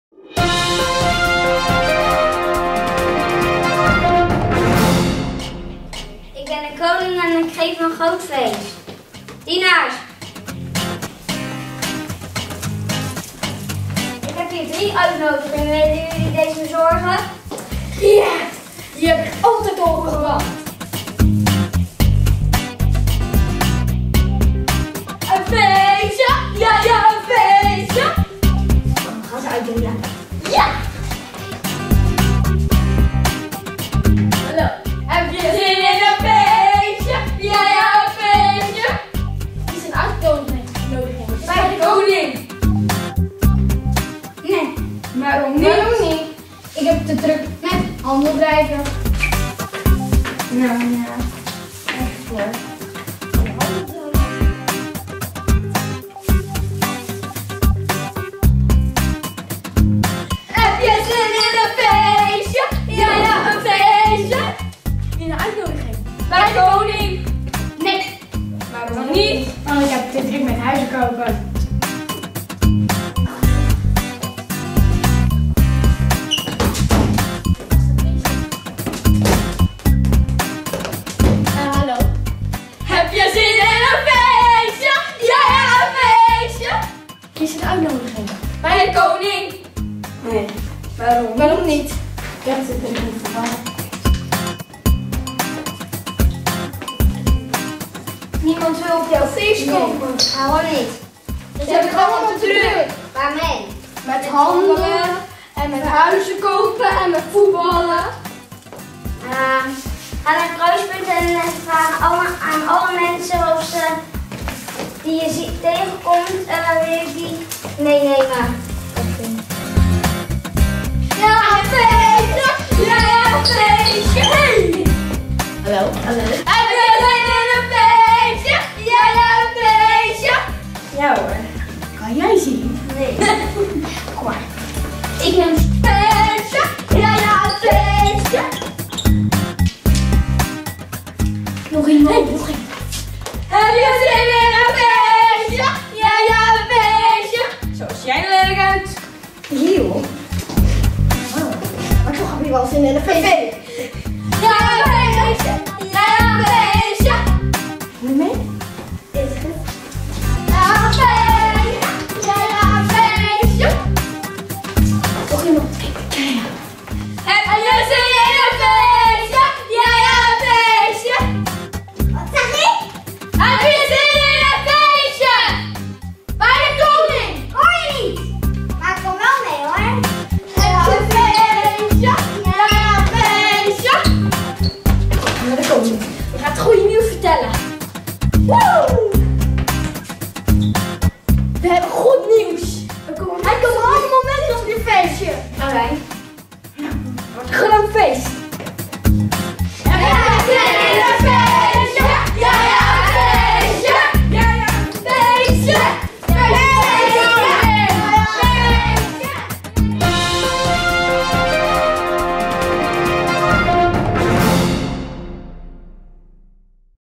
Uitnodiging van de Koning (Lucas 14): 10 jaar vakantieBijbelclub
Dit filmpje is gemaakt door de bovenbouw tijdens het feestweekend van '10 jaar vakantieBijbelclub'. In dit filmpje heeft de bovenbouw het verhaal van Lucas 14, waarin de koning een groot feest heeft georganiseerd en een aantal mensen uitnodigt om te komen maar die stuk voor stuk 'het te druk hebben'. Komt het feest er toch? Kijk zelf maar.